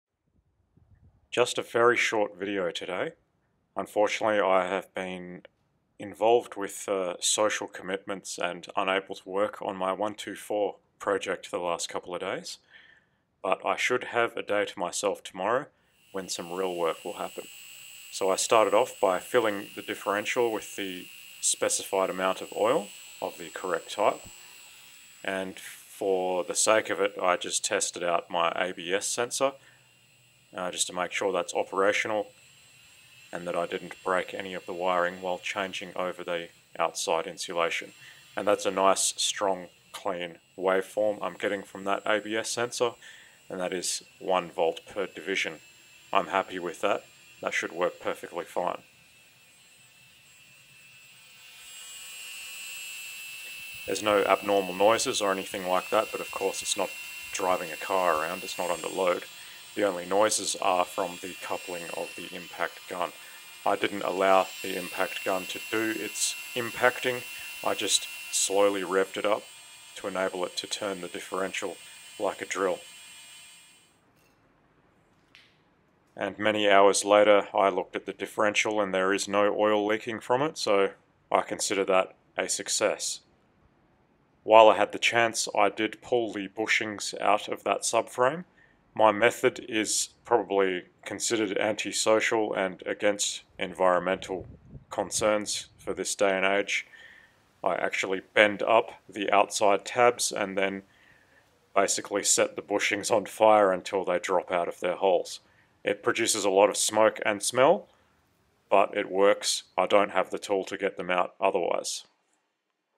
A quick bench test of the W124 differential
This video is about testing the ABS sensor